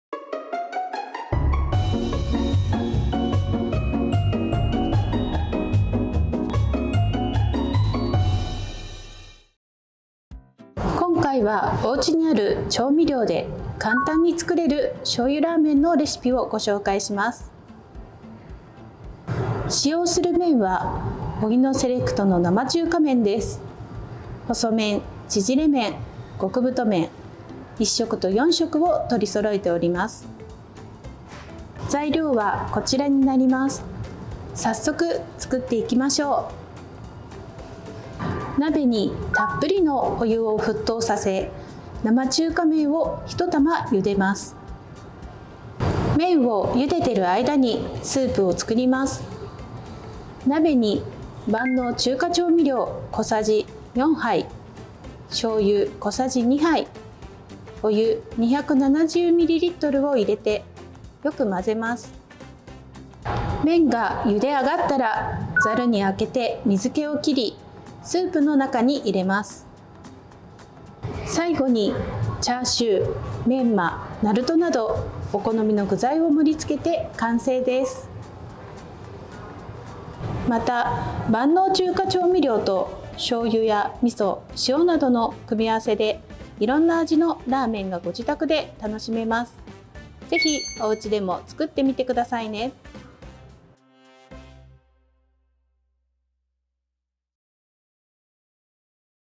【おうちで簡単！】お手軽ラーメンの作り方🍜
今回は、おうちにある調味料で簡単に作れる醬油ラーメンのレシピをご紹介します。

【醤油ラーメン】
■材料(1人前)
・生中華麺(ちぢれ麺)　　　1玉
・チャーシュー　　　　　   2枚
・メンマ　　　　　　　　  10ｇ
・なると　　　　　　　　   1～2枚
・焼のり　　　　　　　　   1枚
・青ねぎ　　　　　　　　   少々
・万能中華調味料　               小さじ4（10ｇ）
・醤油　　　　　　　　       小さじ2
・お湯　　　　　　　　　   270ml

1⃣鍋にたっぷりのお湯を沸騰させ生中華麺を1玉茹でます。
2⃣麺を茹でている間に、スープを作ります。鍋に万能中華調味料小さじ4、
　醤油小さじ2、お湯270mlを入れてよく混ぜます。
3⃣麺が茹であがったら、ザルにあけて水気を切り、スープの中に入れます。
4⃣最後にチャーシュー、メンマ、なるとなどお好みの具材を盛り付けて完成　　です。

※万能中華調味料としょうゆやみそ、しおなどの調味料の組み合わせ
　（1人前あたりの各種味付分量）
　■醤油味：中華調味料小さじ4：醤油小さじ2
    ■味噌味：中華調味料小さじ2：味噌小さじ2
　■しお味：中華調味料小さじ4

色んな味のラーメンがご自宅で楽しめます。
ぜひおうちでも作ってみてくださいね！